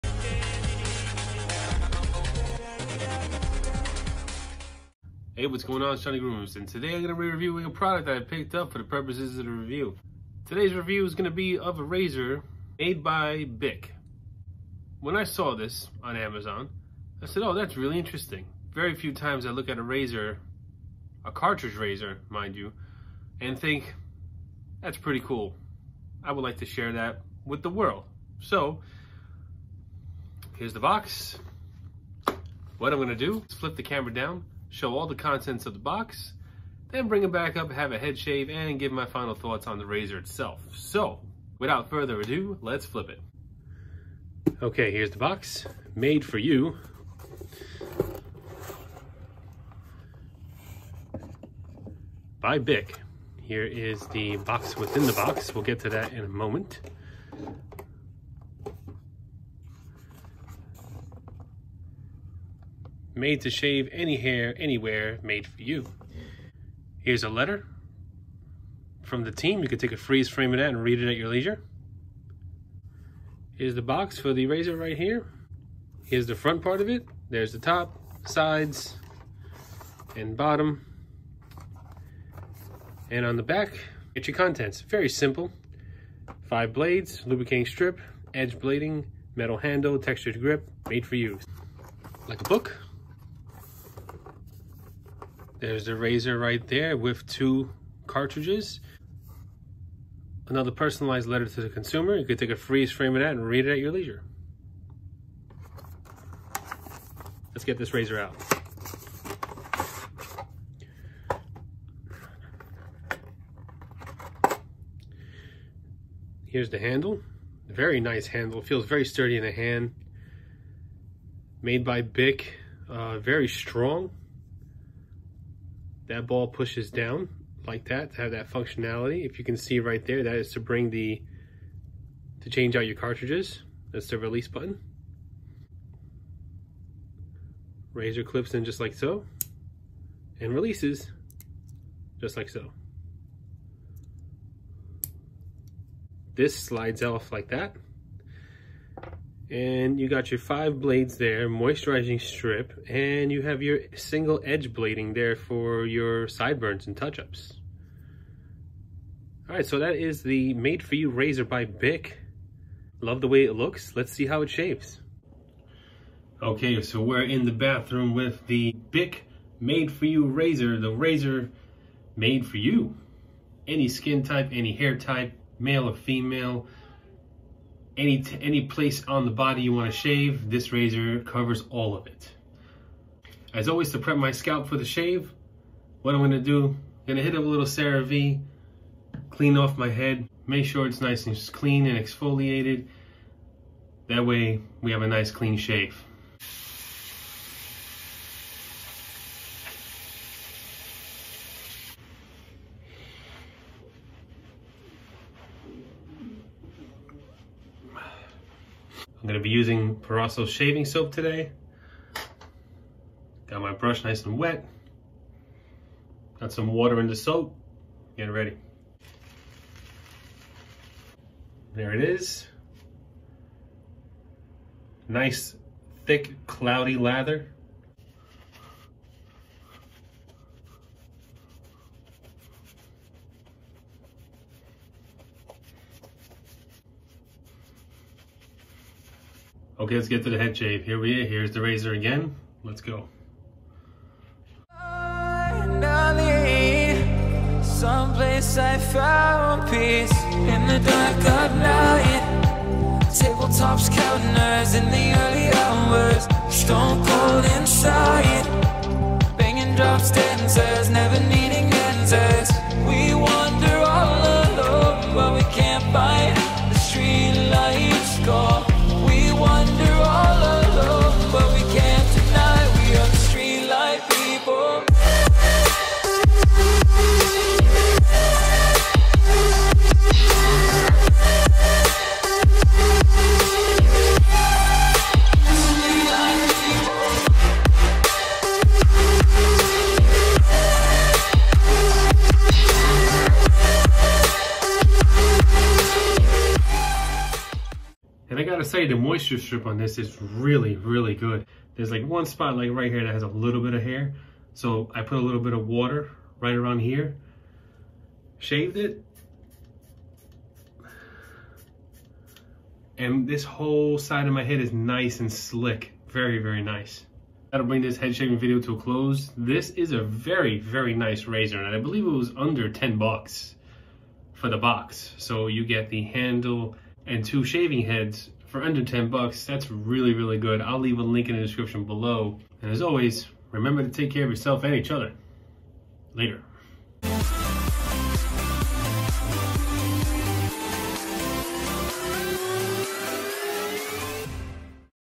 A Razor Made For You - Review
Welcome to the everybodyproject this Razor was created by Bic and was made for you. Regardless of who you are or what you shave. In this review I am shaving my head with the Made For You Razor.
Shaving Soap
Shaving Brush
CeraVe Facial Cleanser